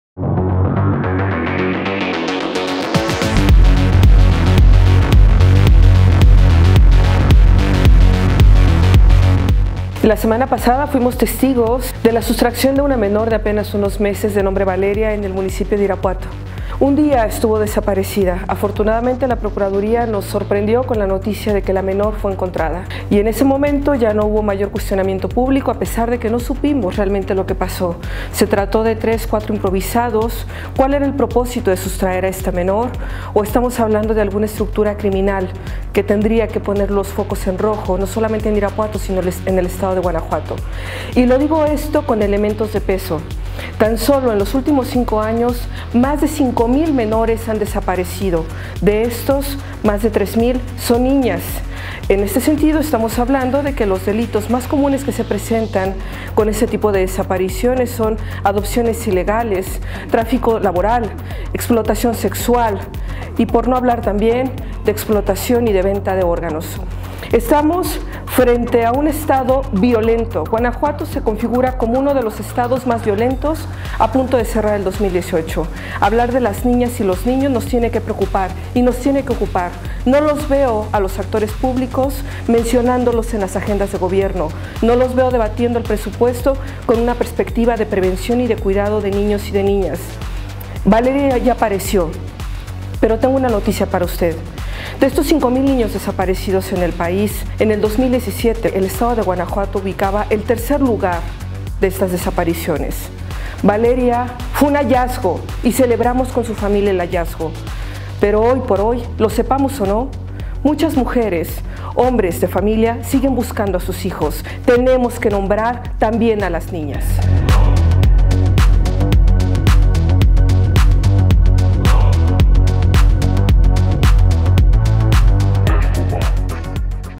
Niñas desaparecidas, ¿dónde están en la agenda de los gobiernos? / Iovana Rocha
El caso Valeria, al que se refiere la activista de derechos de las mujeres, se explica